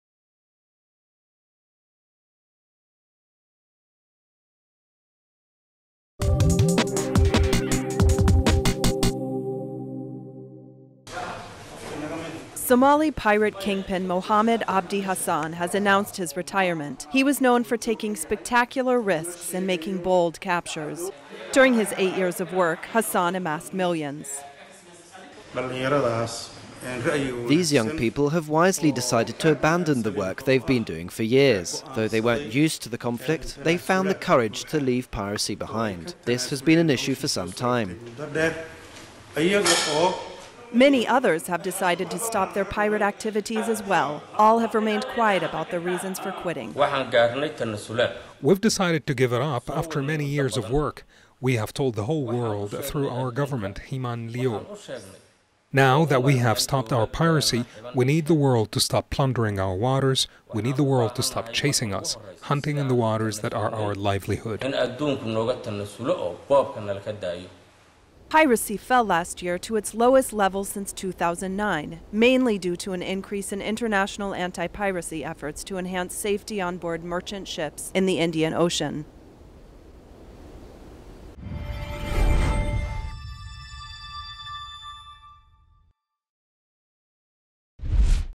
Somalia: Pirates announce retirement
Al Qarra - Somali pirate kingpin Mohamed Abdi Hassan has announced his retirement. He was known for taking spectacular risks and making bold captures. During his eight years of work, Hassan amassed millions of dollars.

Mohamed Abdi Hassan, Somali Pirate

"These young people have wisely decided to abandon the work they've been doing for years. Though they weren't used to conflict, they found the courage to leave piracy behind. This has been an issue for some time."

Many others have decided to stop their pirate activities as well. All have remained quiet about their reasons for quitting.